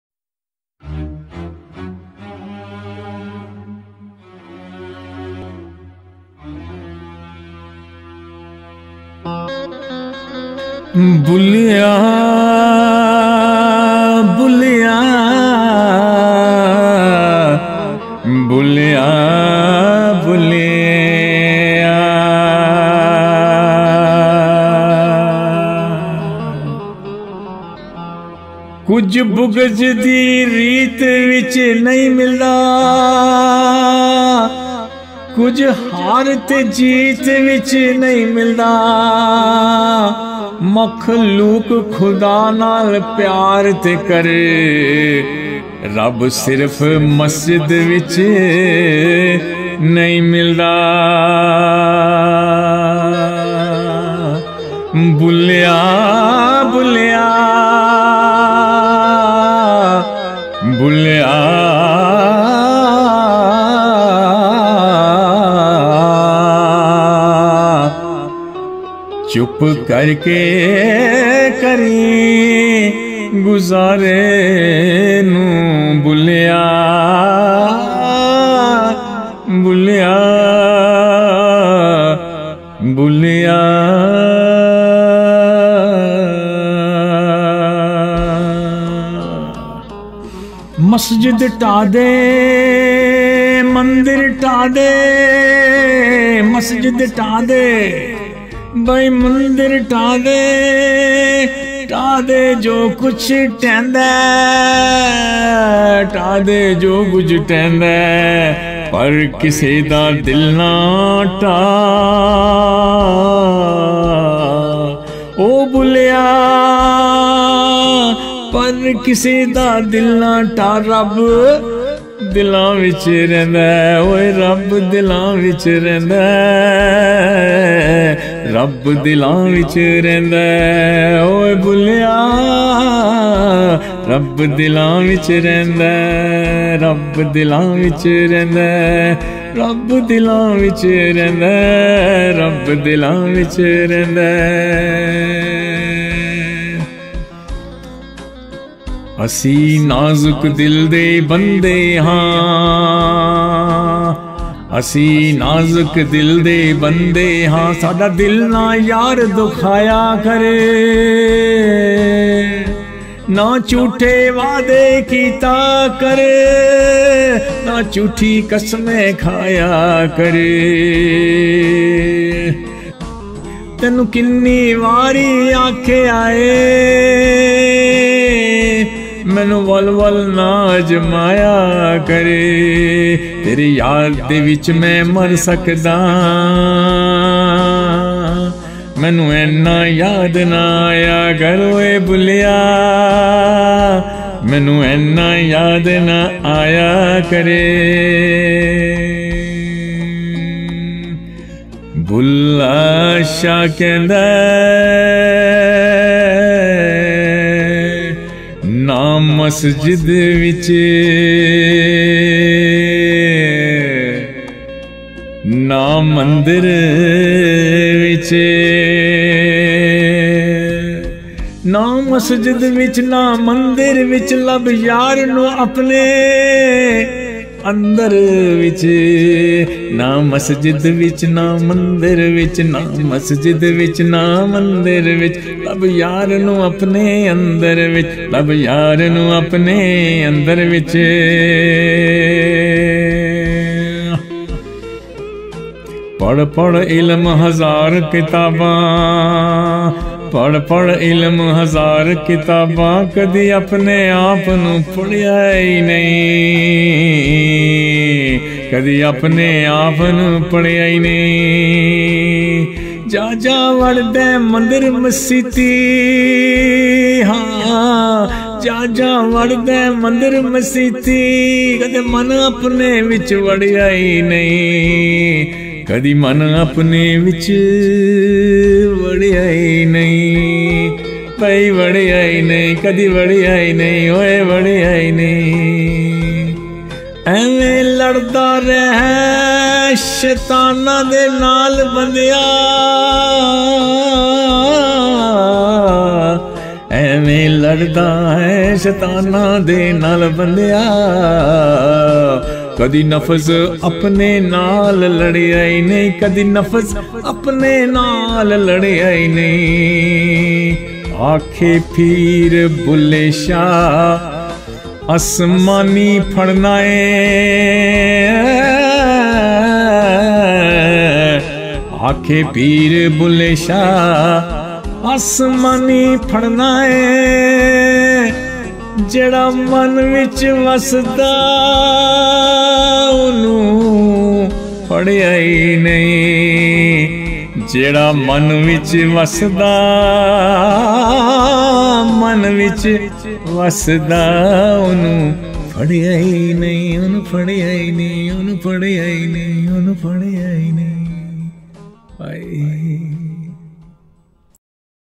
Baba Bulleh Shah Kalam | New quotes added | Inspirational Poetry In Punjabi with meaning in English
Kujh Bughz di Reet vich nai milda
O har te Jeet vich nai milda
Makhlooq-e-Khuda naal pyar te kre
RABB sirf Maseet vich nai milda
Sach sunke log na sehnde ni, sach aakhiye te gal pained ni.
People cannot stand to hear the truth.
They are at your throat if you speak it.
Phir sache paas na behnde ni, sach mitha aashiq pyaare nu.
They keep away from those who speak it.
But truth is sweet to its lovers!
Chup kareke kareen guzaare nu.
Masjid Dha De Mandar Dha De
You can break Mandhir and Masjid
Dhenda Jo Kuch Dha De
You can break whatever else you wish
Ik Banday Da Dil Na Dhawain
Don’t you ever break Human’s HEART
Rab Dilan Wich Rehnda
AS ALMIGHTY Lives in it
Assi Nazuk Dil Dey Lok Han, Sada Dil na Yar Dukhaya Kar,
I am of weak heart please don’t make it sad
Na Jhooty Wady Kitta Kar, Na Jhooti Qasmaan Khaya Kar,
Don’t make false promises and don’t falsely swear on something
Tenu Kinni Wari Me Aakheya Ay, Menu Wal Wal Na Azmaya Kar,
I have told you many times please don’t try these entics
Teri Yad De Vich Mein Mar Ja Saan, Menu Enna Na Yad Aaya Kar…!
I will die in your remembrance Please stop coming in my memory

Na masjid wich Na Mandhir wich
Lab yaar nu apne andhar wich

पढ़ पढ़ आलिम फ़ाज़िल होइओं,
कदे अपने आप नूं पढ़आई नहीं।
जा जा वड़दा एं मन्दिर मसीतीं,
कदे मन अपने विच वड़्याई नहीं ।
ऐवें रोज़ शैतान नाल लड़नै,
कदे नफ़स अपने नाल लड़्याई नहीं ।
बुल्ल्हे शाह असमानीं उडदियां फड़नै,
जेहड़ा घर बैठा उहनूं फड़्याई नहीं ।